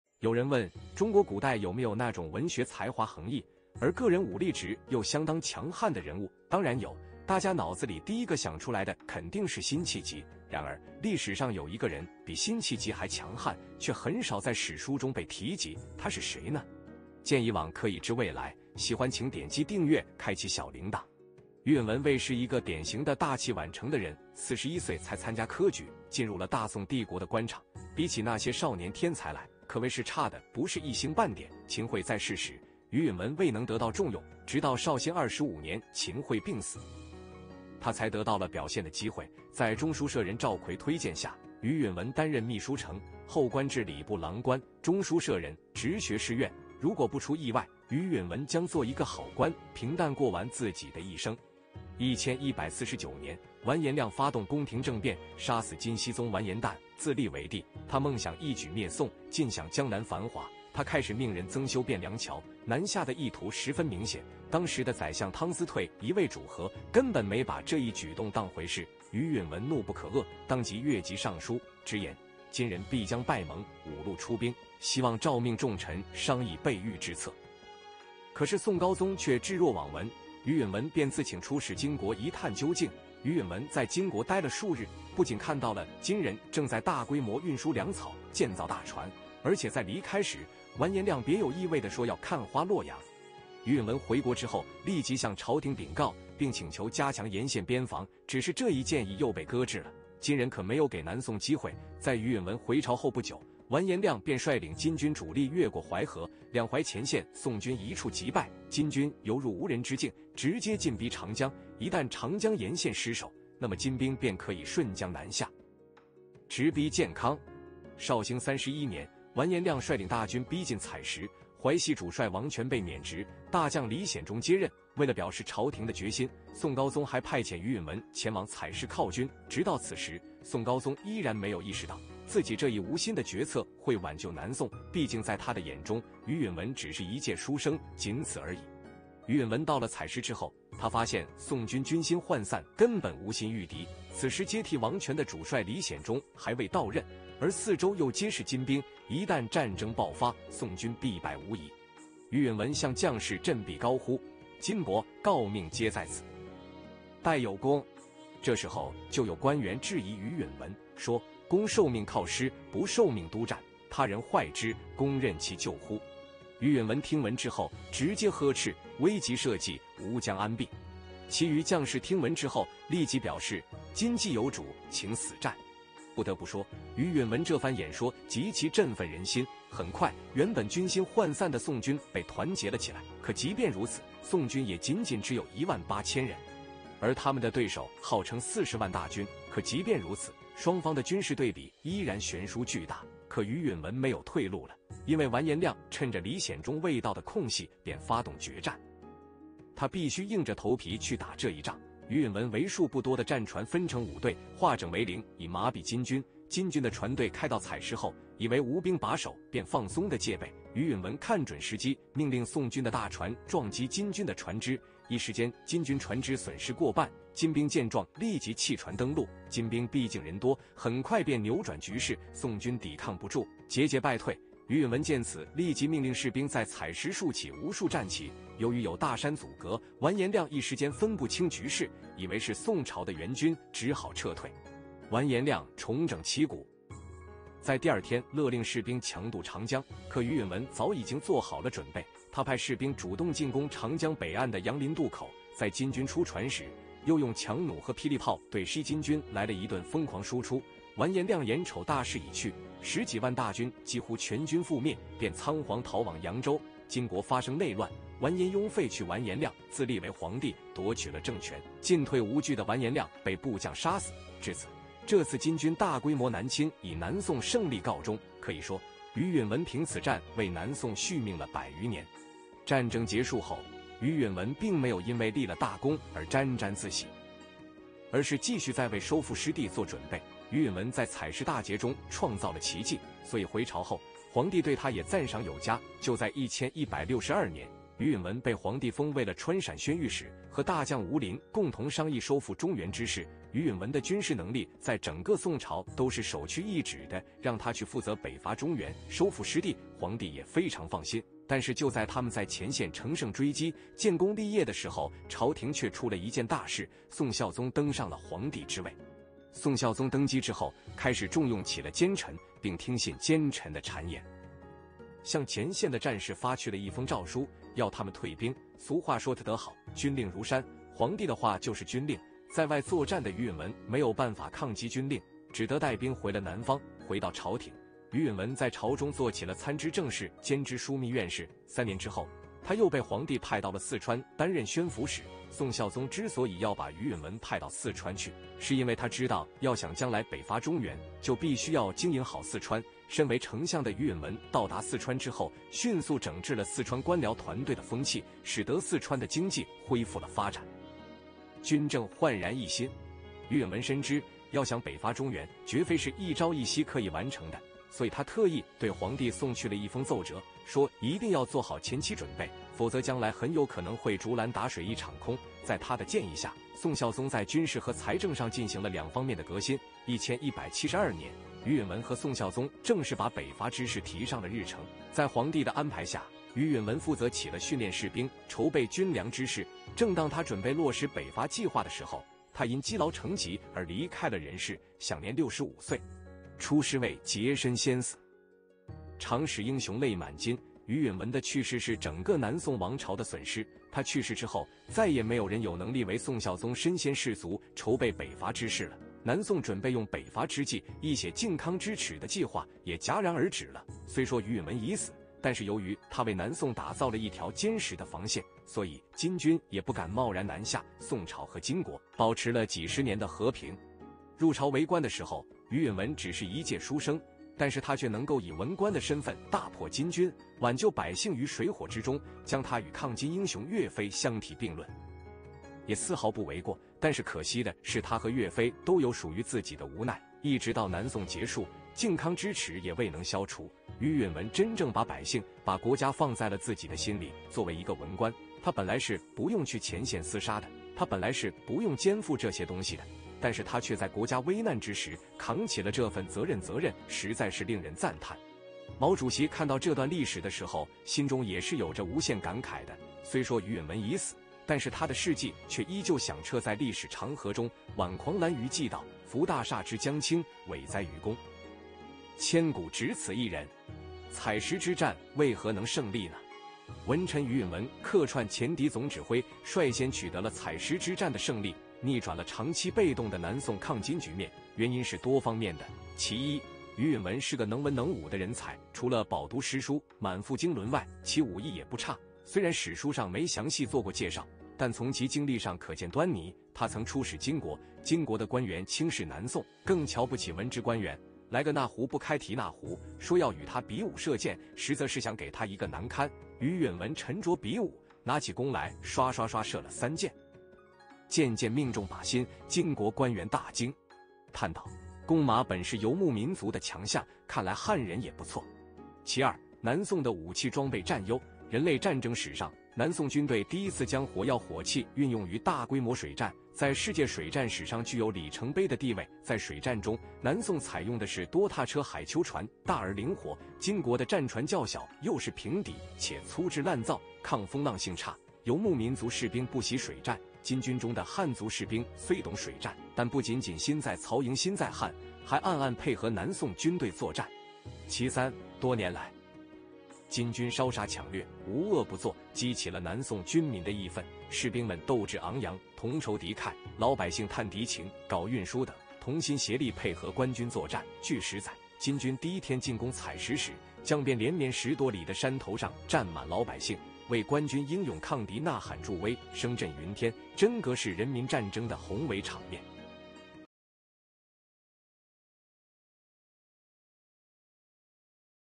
書生會武功誰也擋不住，大宋最強書生，比辛棄疾還能打，以2萬人擊敗40萬敵軍【鑒史禦今】不爲人知的曆史
有人問，中國古代有沒有那種文學才華橫溢，而個人武力值又相當強悍的人物？當然有，大家腦子裏第壹個想出來的肯定是辛棄疾。然而，曆史上有壹個人，比辛棄疾還強悍，卻很少在史書中被提及，他是誰呢？不爲人知的曆史